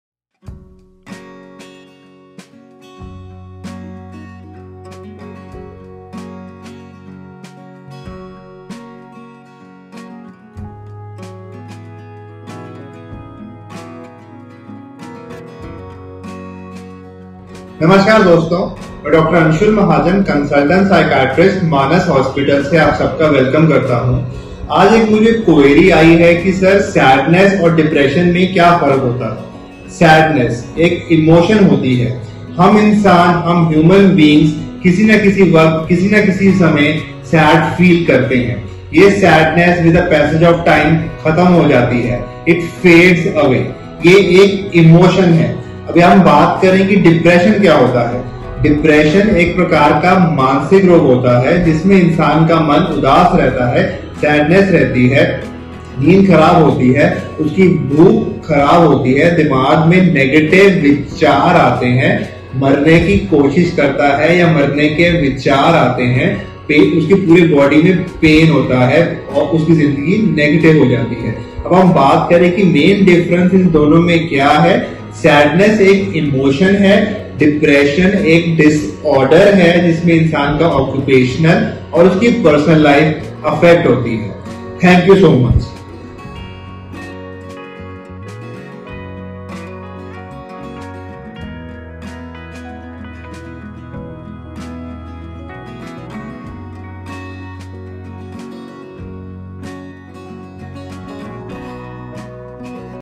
What is the Difference between Sadness vs Depression? Dr. Anshul Mahajan | Manas Hospital
Sadness and Depression are terms that are associated with each other. Depression problem can lead the different types of health problems such as sexual problems. As you see in this video Dr. Anshul Mahajan the best Psychiatrist in Ludhiana is providing detailed information regarding sadness and depression problems. You can watch this complete video and can get detailed information about the depression problem. Also can book appointment for initial consultations.

📌 Manas Hospital: Psychiatrists in Ludhiana